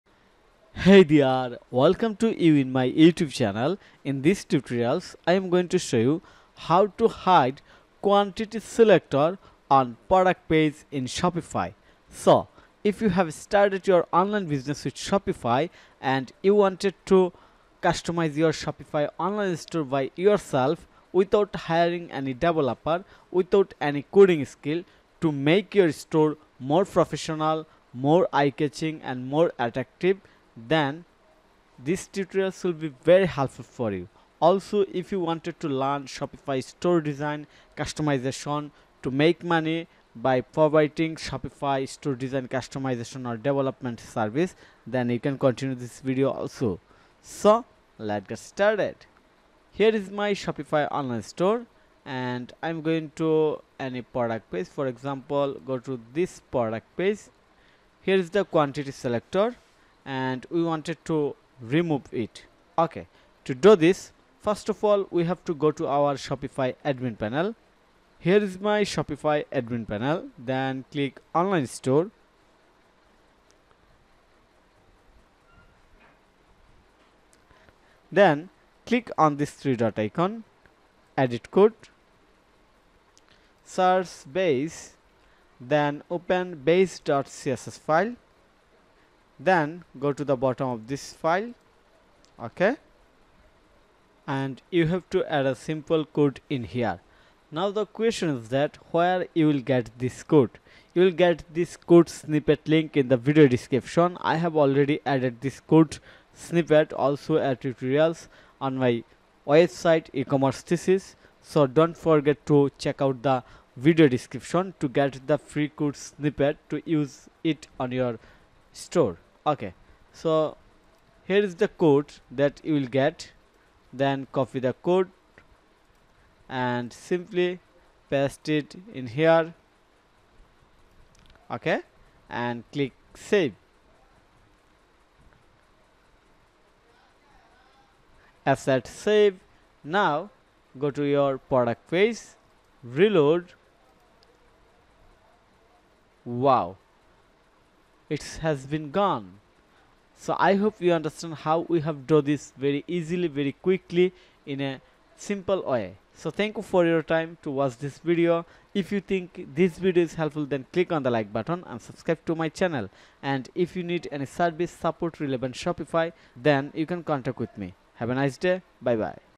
How To Hide Quantity Selector On Product Page In Shopify ✅ Shopify Product Page Customize
💻 Welcome to * How To Hide Quantity Selector On Product Page In Shopify ✅ Shopify Product Page Customize  *

📝 Video Description: Want to create a more streamlined and minimalist product page on your Shopify store? Learn how to easily hide the quantity selector and enhance the overall user experience. In this tutorial, we'll guide you through the step-by-step process of customizing your Shopify theme to remove the quantity selector from your product pages. Whether you're using a custom theme or a popular template, this video will show you how to make this change with ease.